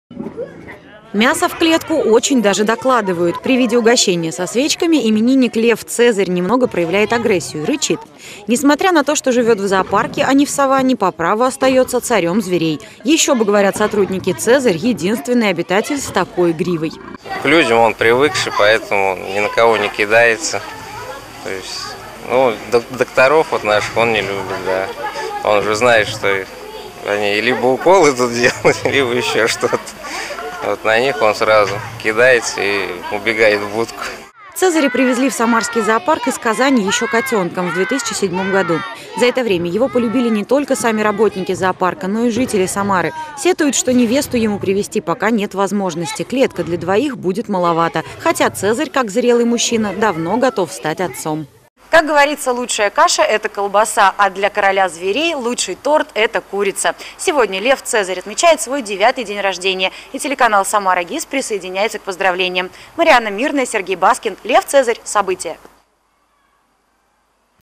В самарском зоопарке лев Цезарь отмечает свой 9-ый день рождения
Как на Цезаревы именины - приготовили торт из курицы. 9 тушек - по одной за каждый прожитый год жизни. В Самарском зоопарке царь зверей принимает поздравления с днём рождения